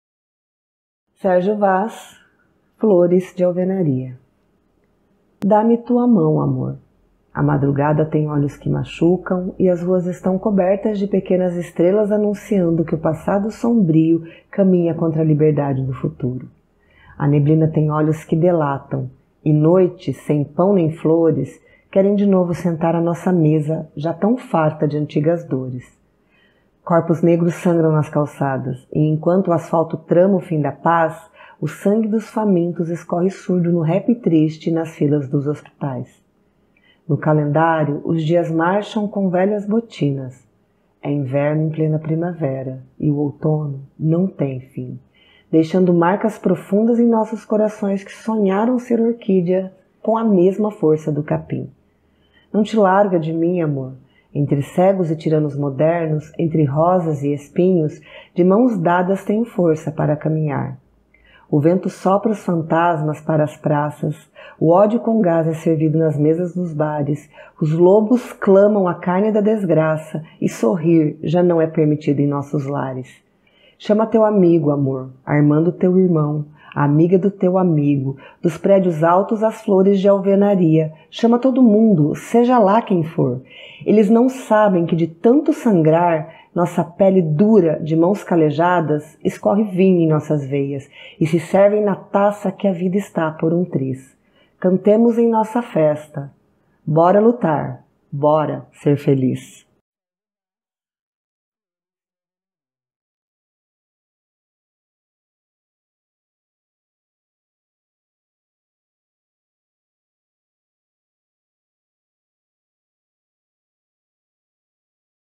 Sérgio Vaz: Flores de Alvenaria |Recitando Poema |Poesia Brasileira Declamada Poeta Atual Literatura
Poema de Sérgio Vaz Flores de Alvenaria, declamado por Moema Andreia, para o Podcast de Poesias declamadas Toma Aí Um Poema. Sérgio Vaz é um poeta brasileiro. Promoveu em 2007 a Semana de Arte Moderna da Periferia, inspirada na Semana de Arte Moderna de 1922. Criou outros eventos, como a Chuva de Livros; o Poesia no Ar, em que papeis com versos são amarrados a balões de gás e soltos no ar; e o A joelhaço, em que homens se ajoelham na rua para pedir perdão às mulheres no Dia Internacional da Mulher. Foi escolhido pela revista Época um dos 100 brasileiros mais influentes de 2009. Nasceu em Minas Gerais em 1964, atualmente tem 56 anos.

Declamação de Poesia Brasileira: Poeta da Periferia.

►►► Por 5,99, aceita uma Seleta de Sonetos para Acompanhar? Apoie este projeto! Saúde!

Poema: Flores de Alvenaria
Poeta: Sérgio Vaz
Voz: Moema Andreia

Use
Siga @tomaaiumpoema

"Da-me tua mão amor
a madrugada tem olhos que machucam
e as ruas estão cobertas de pequenas estrelas
anunciando que o passado sombrio
caminha contra a liberdade do futuro.

A neblina tem olhos que delatam
e noites sem pão nem flores
querem de novo sentar à nossa mesa
jã tão farta de antigas dores.

Corpos negros sangram nas calçadas
e enquanto o asfalto trama o fim da paz
o sangue dos famintos escorre surdo
no rap triste e nas filas dos hospitais.

No calendário os dias marcham com velhas botinas
é inverno em plena primavera, e o outono não tem fim
deixando marcas profundas em nossos corações
que sonhou ser orquídia com a mesma força do capim.

Não te larga de mim amor
entre cegos e tiranos modernos
entre rosas e espinhos
de mãos dadas tenho força pra caminhar.

O vento sopra os fantasmas para as praças
o ódio com gás é servido nas mesas dos bares
os lobos clamam a carne da desgraça,
e sorrir já não é permitido em nossos lares.

Chama teu amigo amor
a irmã do teu irmão
a amiga do teu amigo
do prédios altos às flores de alvenaria
chama todo mundo
seja lá quem for.

Eles não sabem que te tanto sangrar
Nessa pele dura de nossas mãos calejadas
escorre vinho em nossas veias
e se servem na taça que a vida está por um triz

cantemos em nossa festa:
bora lutar
bora ser feliz."

►► Que tal ter um poema seu aqui? É só preencher o formulário!
Após o preenchimento, nossa equipe entrará em contato para informar a data agendada.